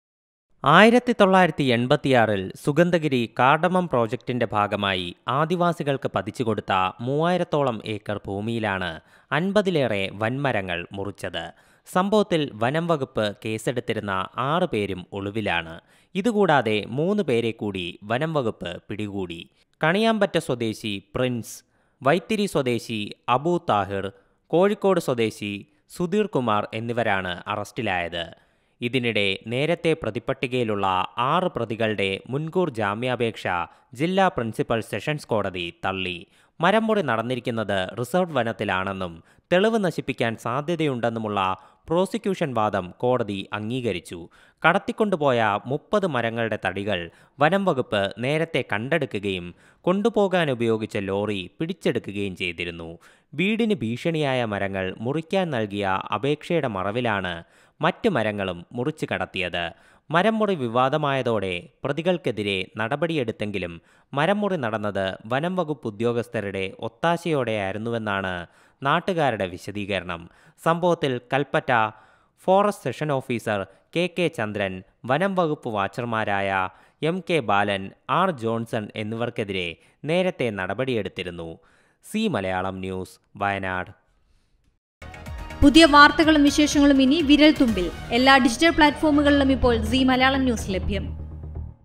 Sugandhagiri Tree Felling | മരംമുറിക്കേസിൽ പ്രതിപ്പട്ടികയിലുള്ള 6 പേരെ കൂടാതെയാണ് 3 പേർ പിടിയിൽ
Sugandhagiri Tree Felling | സുഗന്ധഗിരി മരംമുറിക്കേസിൽ പ്രതിപ്പട്ടികയിലുള്ള 6 പേരെ കൂടാതെയാണ് മൂന്ന് പേർ കൂടി പിടിയിൽ